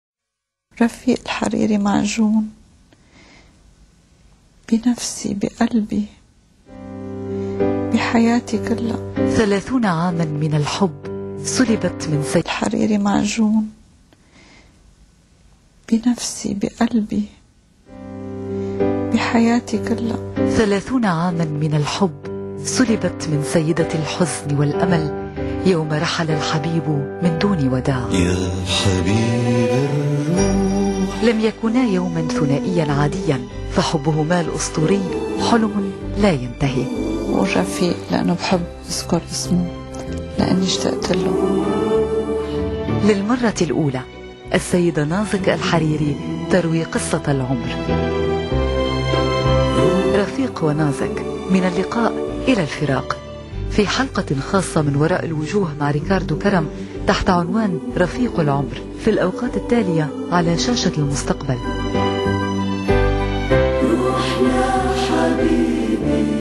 Rafi 2el 3omor - Life Companion
ad about Ricardo Karam's Interview with Mrs. Nazek Hariri about her life with late PM Rafic Hariri